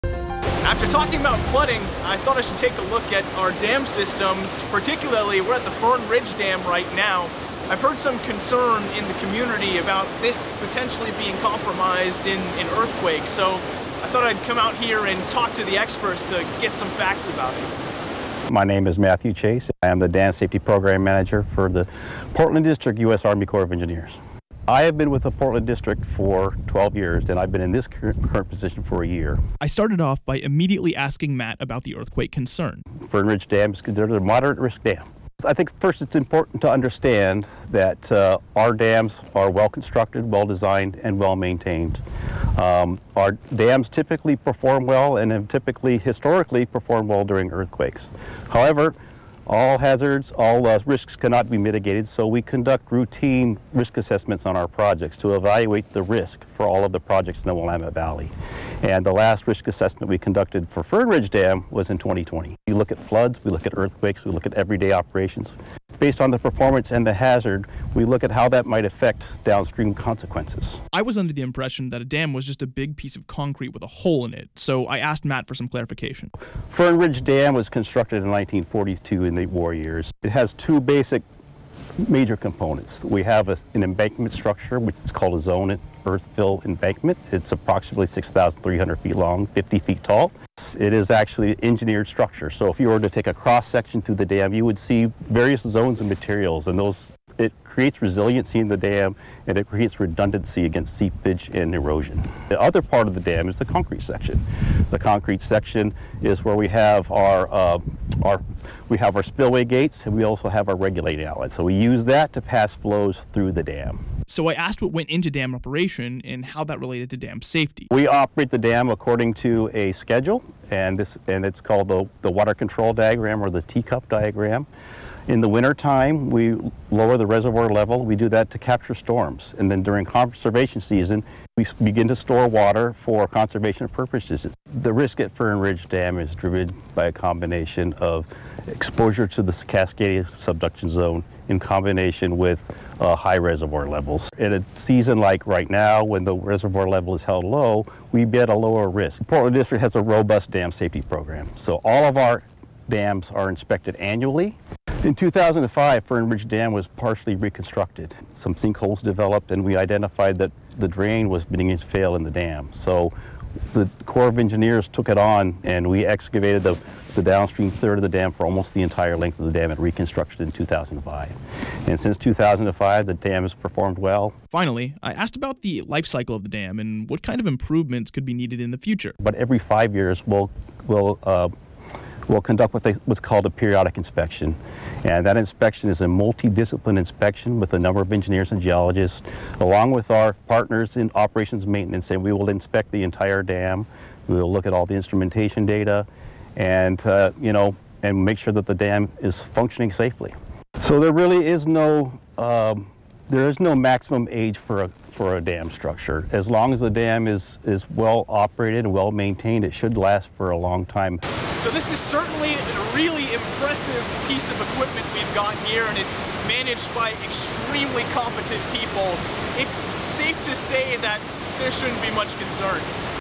Are Oregon's dams ready for an earthquake?
Officials said many of Oregon's dams can resist an earthquake, but they test frequently to make sure no flaws have appeared.